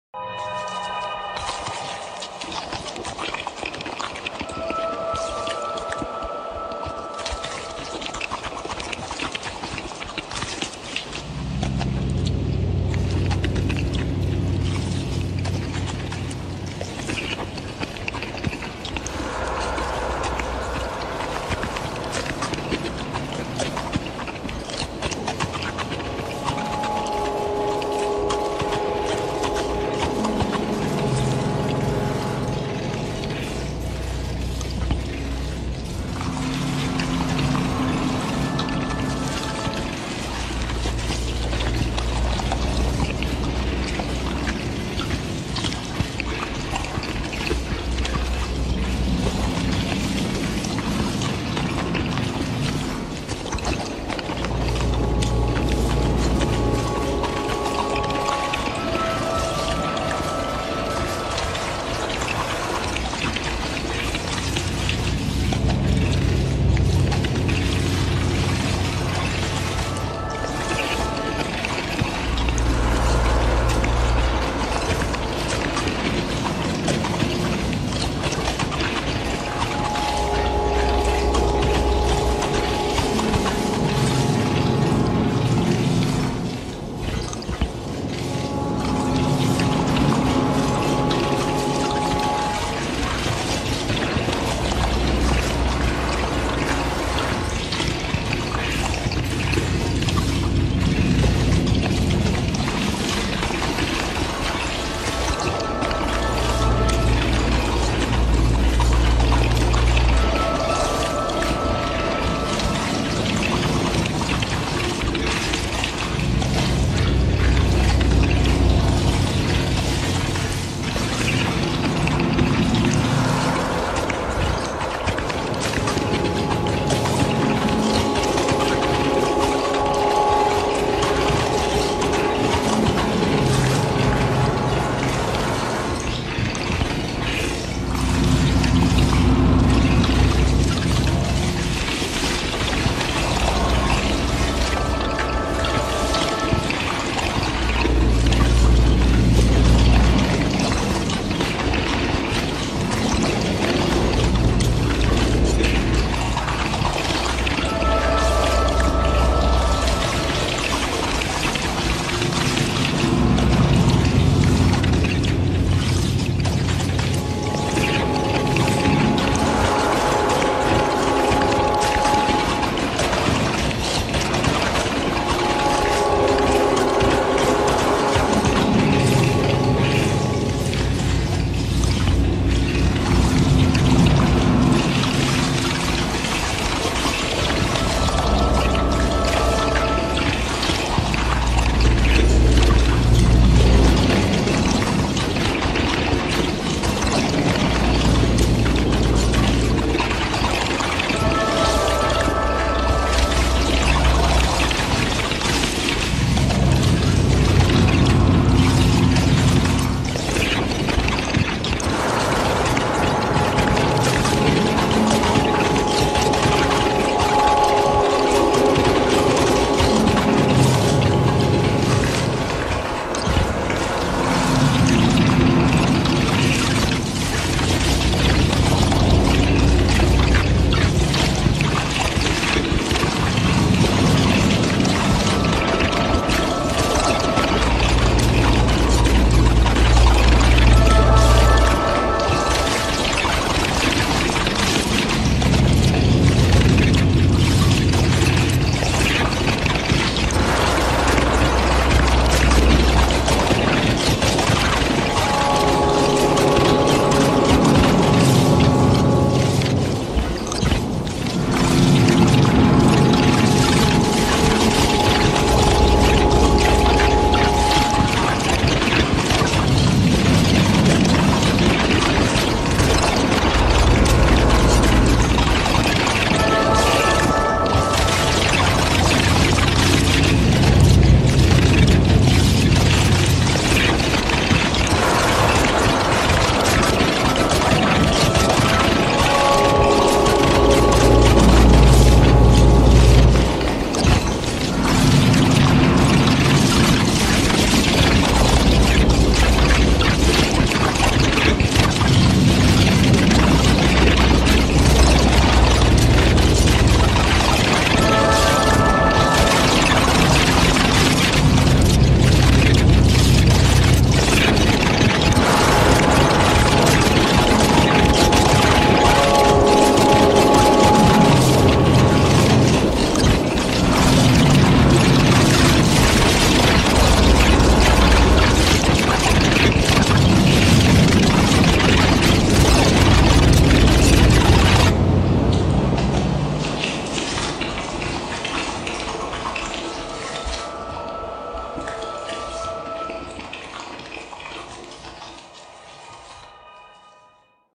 THE DARK LORD WORKS IN MYSTERIOUS WAYS
i decided to try to make some random person's cursed video, even longer and more chaotic. i currently do not have direct permission from Totally Real Rye, but i also am not even sure if they created this footage either or if they just ripped it and added spooky music?